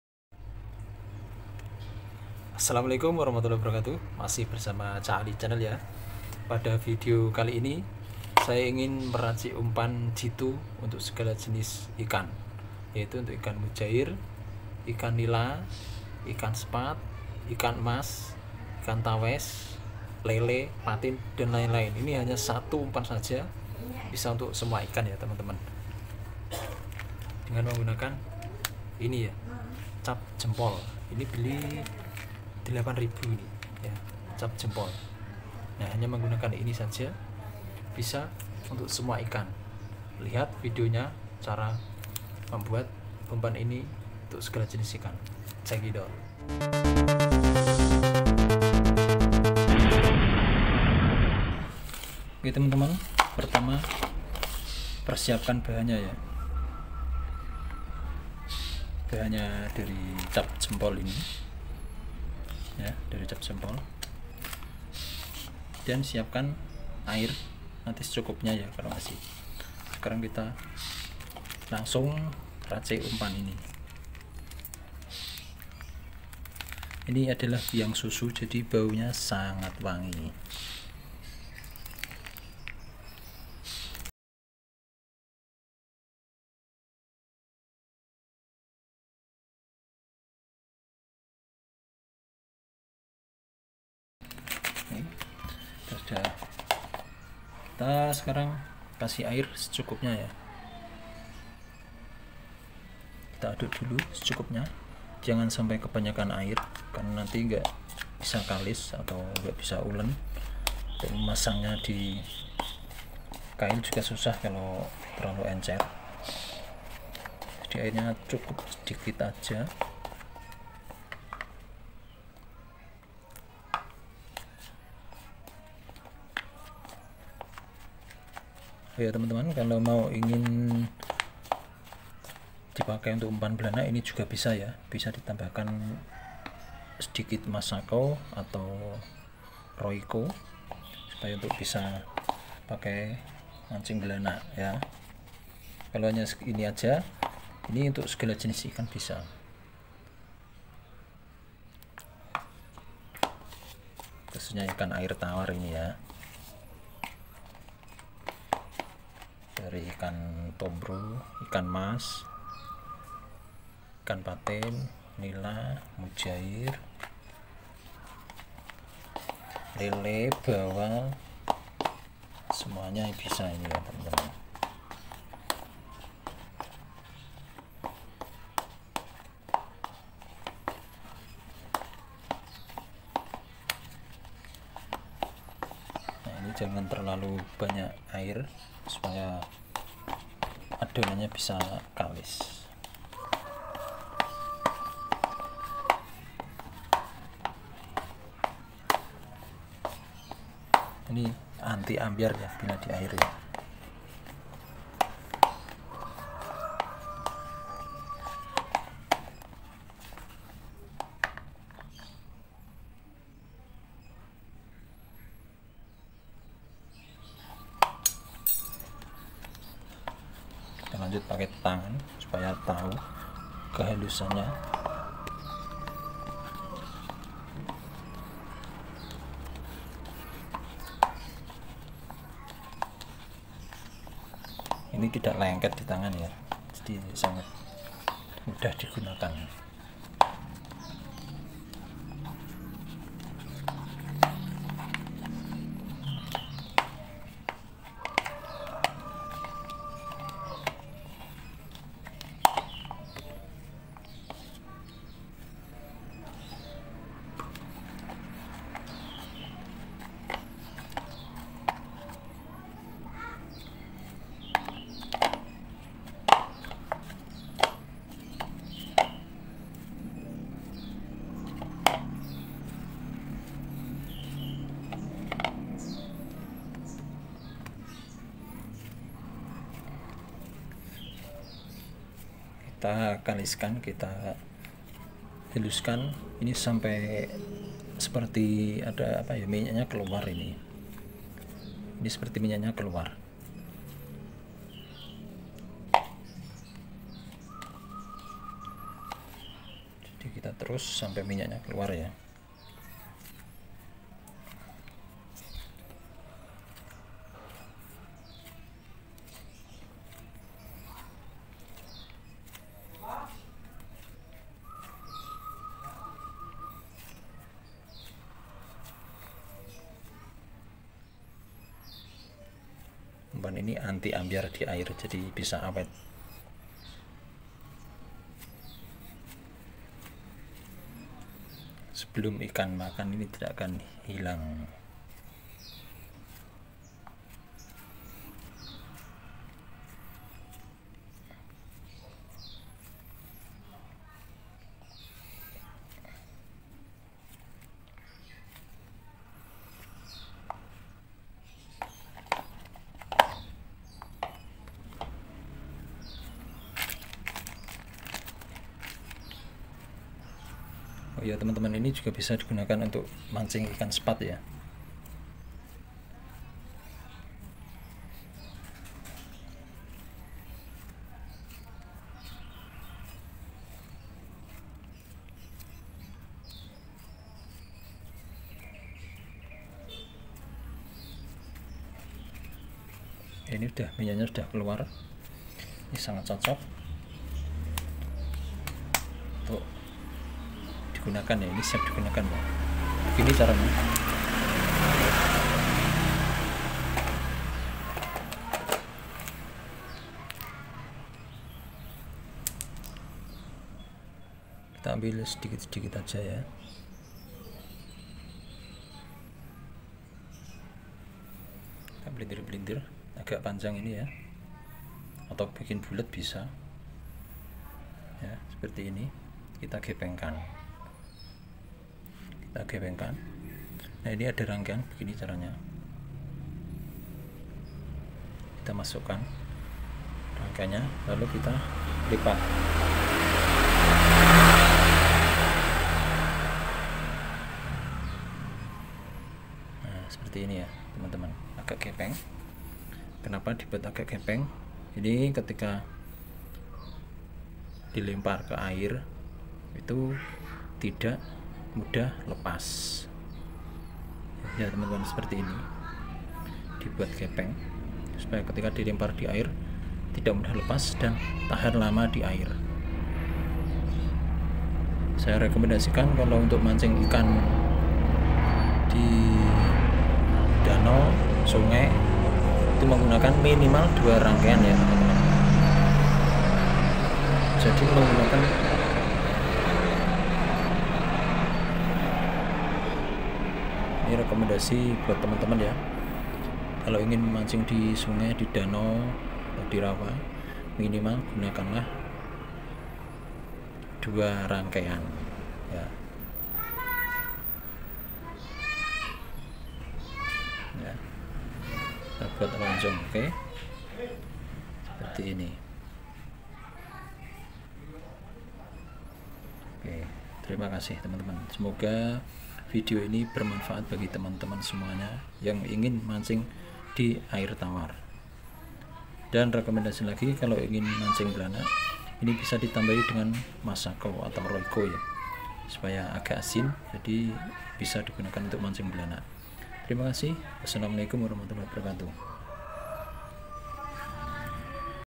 UMPAN NILA // UMPAN MUJAIR //UMPAN ikan Mas// UMPAN JITU //UMPAN AMPUH IKAN AIR TAWAR
hallo Sobat mancing
pada video kali ini saya akan berbagi umpan untuk segala ikan air tawar...
hanya satu umpan ini saja semua ikan air tawar akan bisa didapatkan
.simak video lengkapnya ya
semoga bermanfaat
terimakasih